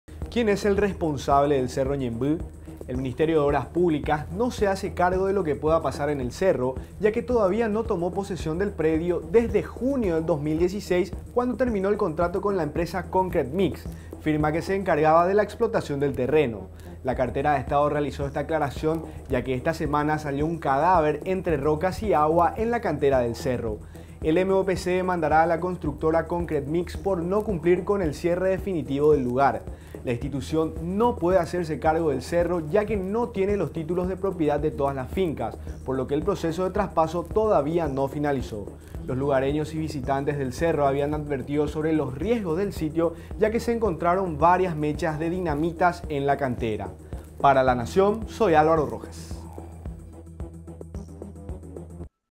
Cerro Ñemby: Demanda por incumplimiento del cierre
¿Quién es el responsable del Cerro Ñemby? El ministerio de Obras Públicas no se hace cargo de lo que pueda pasar en el cerro, ya que todavía no tomó posesión del predio desde junio de 2016 cuando terminó el contrato con la empresa Concret Mix.